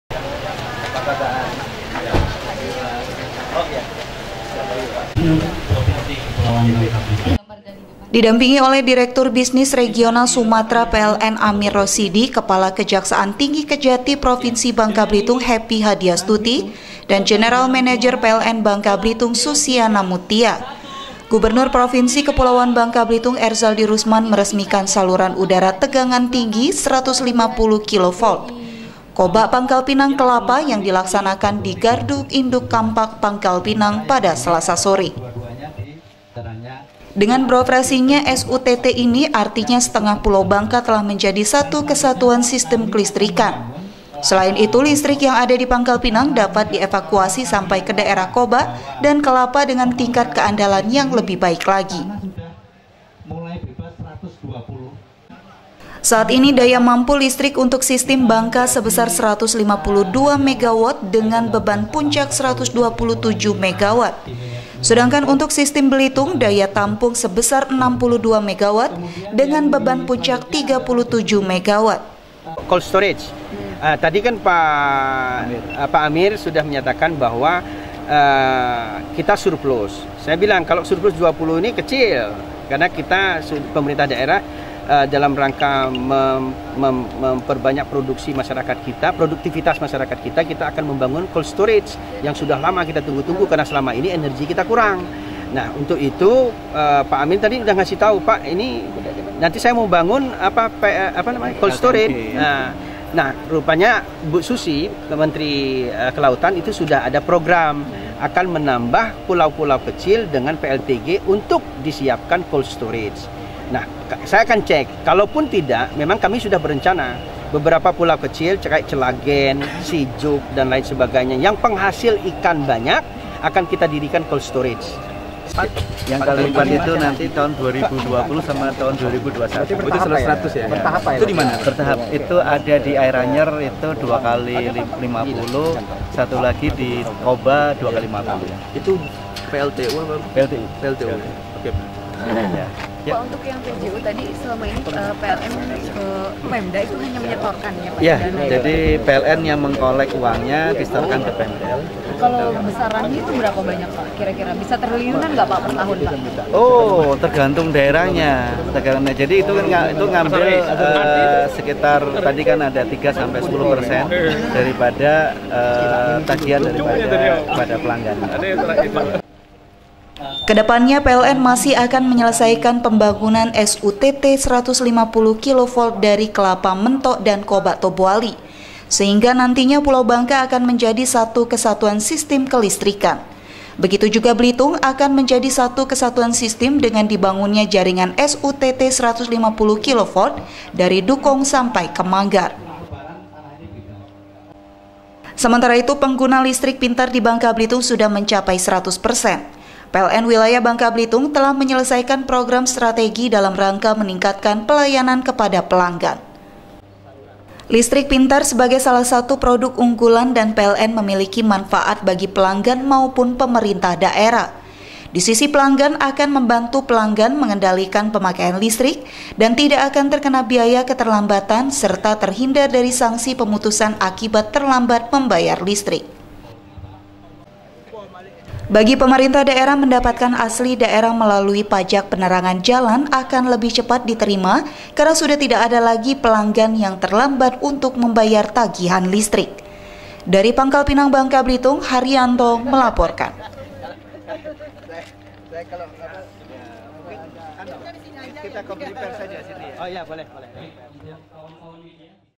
GUBERNUR RESMIKAN SUTT 150KV KOBA-PANGKALPINANG-KELAPA
Program strategis untuk menciptakan kenyamanan bagi masyarakat, terus dilakukan Perusahaan Listrik Negara(PLN).

Di Bangka Belitung, selasa sore kemarin (20/06/2017) gubernur provinsi Kepulauan Bangka Belitung, Erzaldi Roesman, meresmikan Saluran Udara Tegangan Tinggi (SUTT) 150 kv Koba-Pangkalpinang-Kelapa. Dengan beroperasinya SUTT tersebut, diharapkan bisa memberikan solusi yang tepat untuk mengatasi bayer pet.